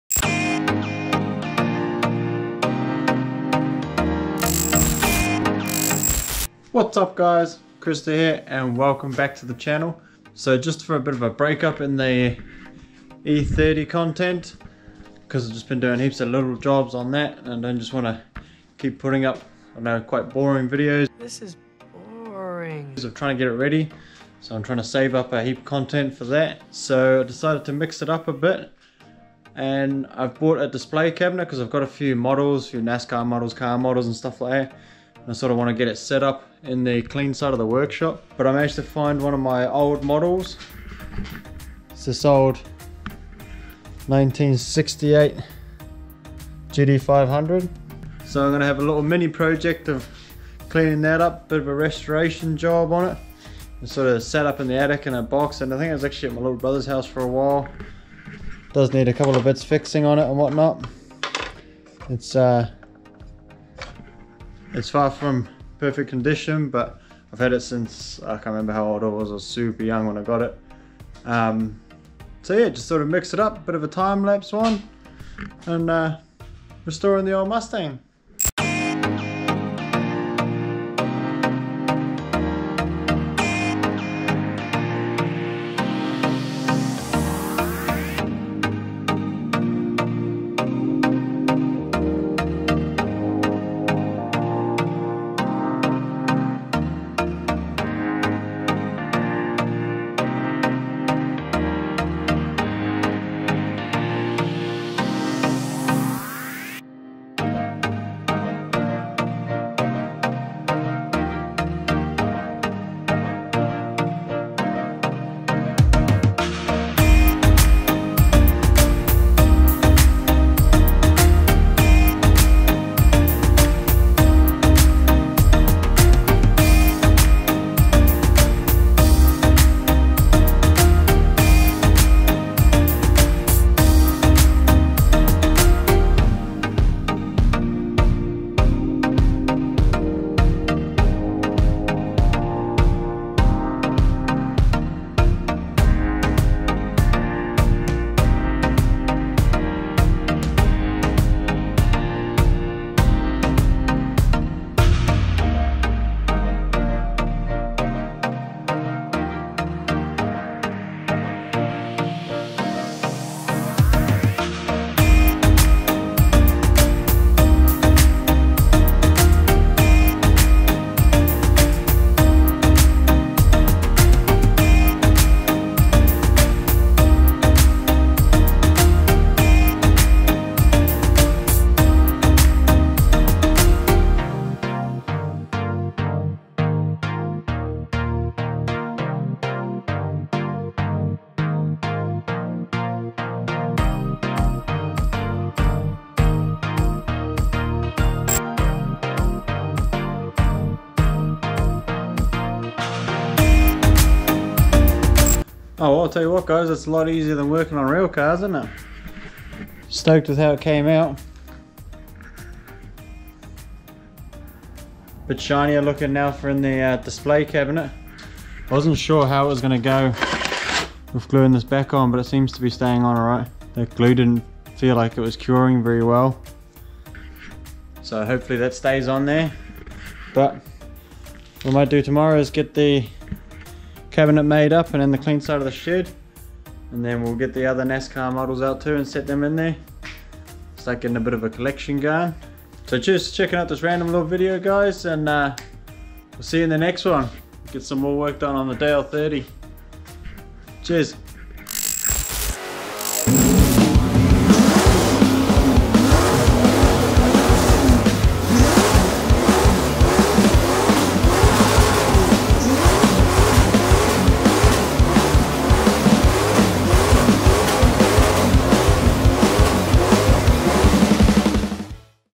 1968 SHELBY GT500 // MODEL RESTORATION
A bit of a change up in content with this one!
With work on the DALE30 waiting for parts I figured I would pull out some of my old models and get them ready for the new display cabinet we bought for the workshop.
I've had this gem of a model since my 12th birthday, so around 18 years.
But it was neglected for most of those years, just the usual got distracted by real cars, beers and girls.

But I'm stoked how it turned out!

Cheers for checking out this video! be sure to check out some of my other content!